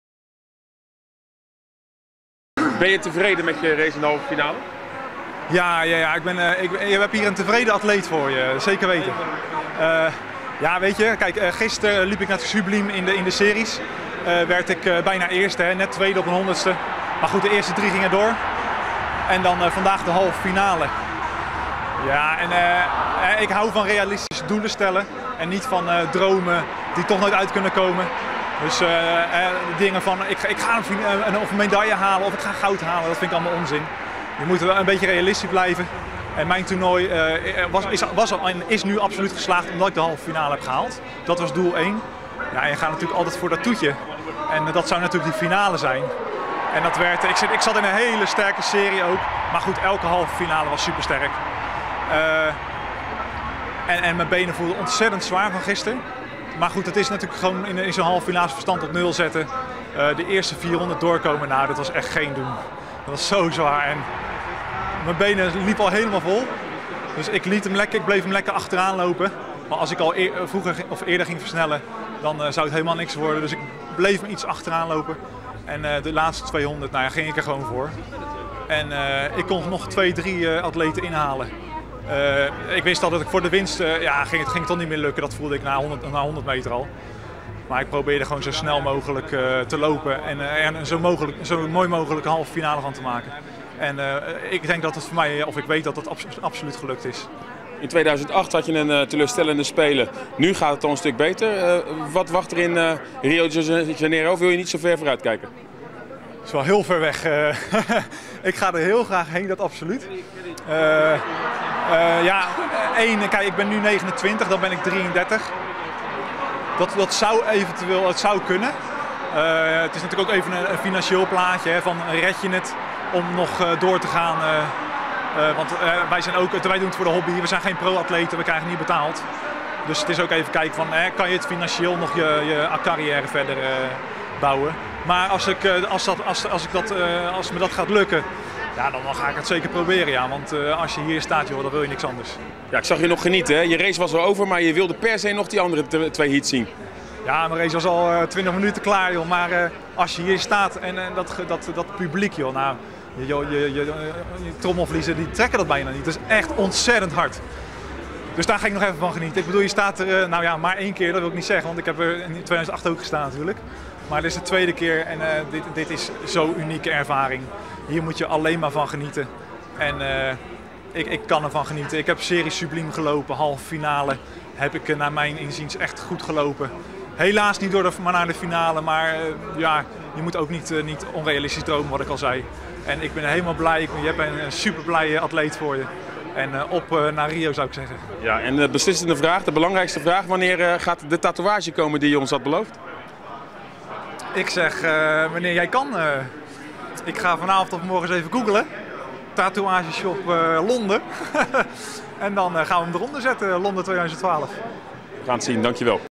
Robert Lathouwers in gesprek met Sinclair Bischop
De halve finale van de 800 meter was dinsdag het eindstation voor Robert Lathouwers op de Olympische Spelen in Londen. De vijfde plaats in 1.45,85 was niet goed genoeg voor de 29-jarige atleet om zich te plaatsen voor de finale.

   Lees meer: Lathouwers uitgeschakeld op Spelen